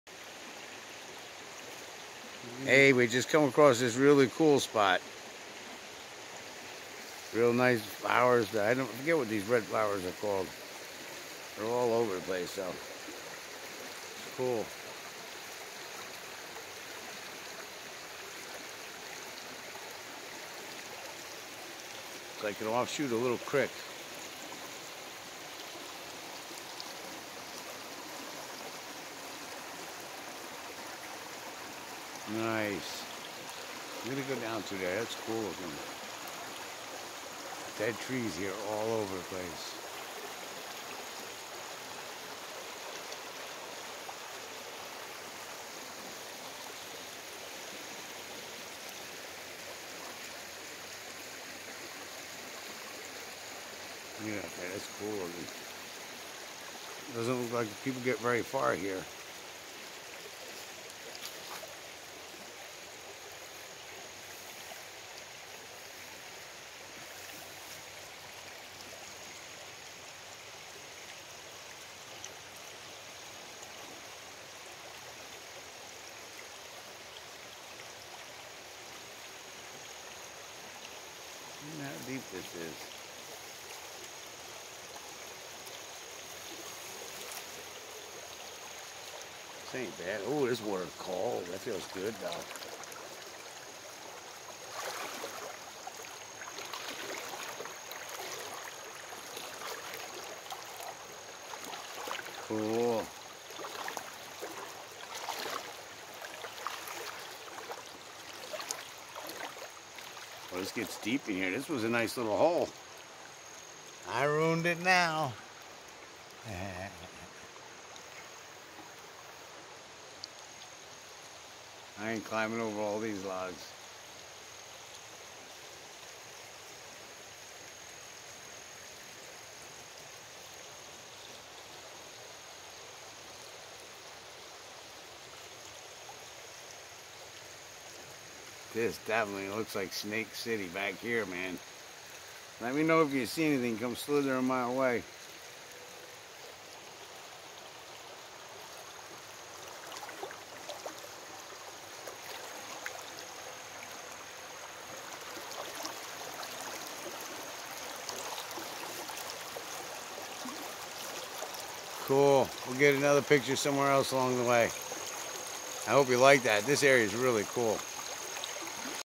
Mongaup creek
we followed the path and it shot off to the side with this Creek that led right back into the main river